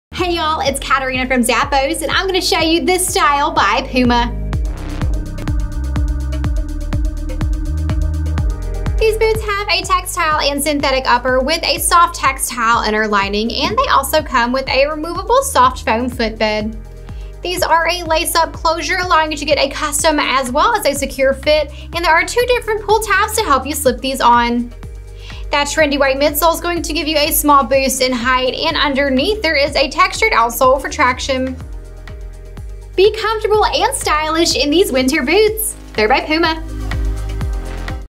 PUMA Adela PURETEX Winter Boot SKU: 9504385
With its highly functional, breathable and waterproof upper, you'll certainly make no mistakes this season wearing the nylon PUMA&reg; Adela PURETEX Winter Boot with a lace-up closure.
Heel tab with a soft faux-fur lining and the Softfoam comfort footbed that provides cushioning with every step.
Imported.